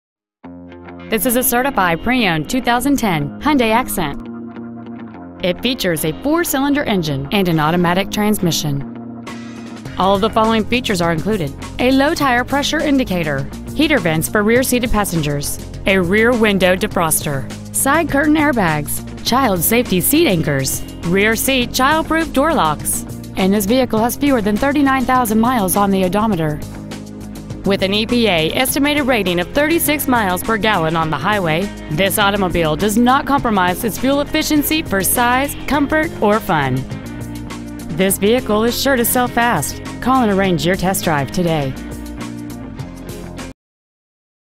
2010 HYUNDAI ACCENT Miami FL
Year : 2010
 Make : HYUNDAI
 Model : ACCENT
 Engine : dohc 16v i-4
 Trans . : automatic
 Exterior : white
 Miles : 38,967
 Interior : white

 Lehman Mazda

 2010 HYUNDAI ACCENT Miami FL